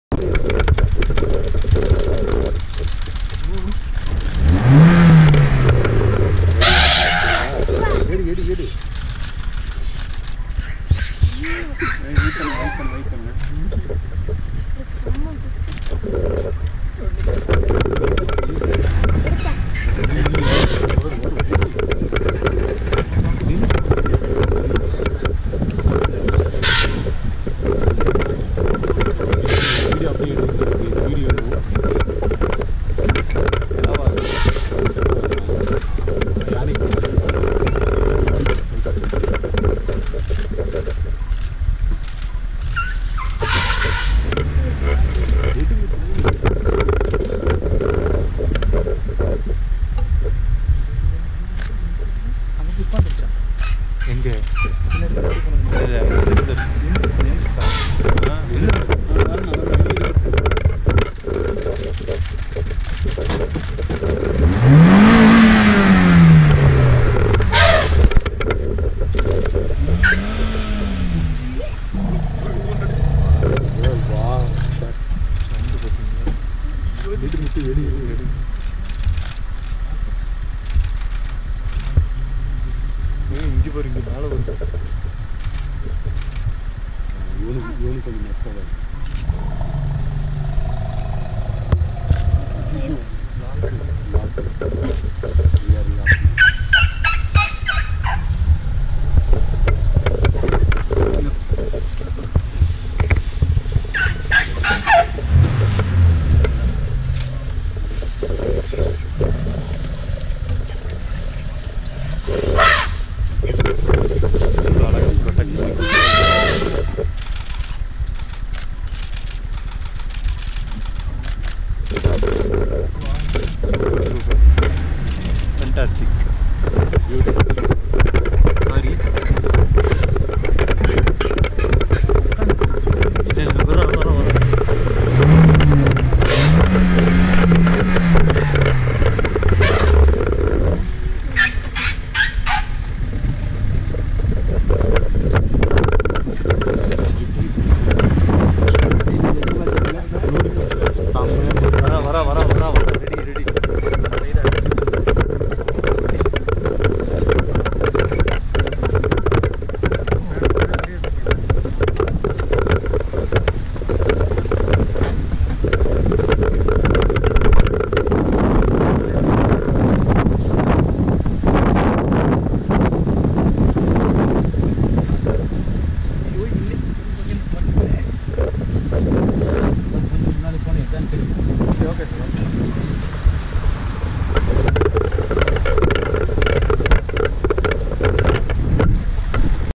Elephant chase in bandipur during safari | Sathish Ramamoorthy
Elephant chase in bandipur tiger reserve during safari on October 2020.